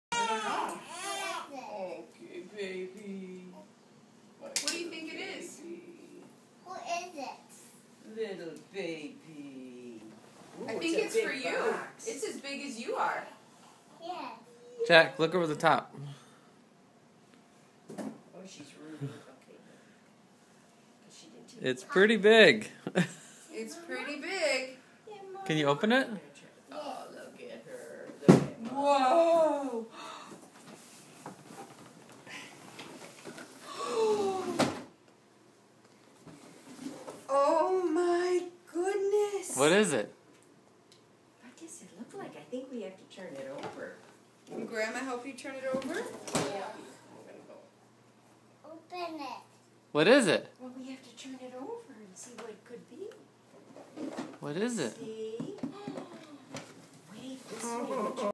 New Truck from Grandpa Norm & Grandna Lynda
Jack gets a new Bruder truck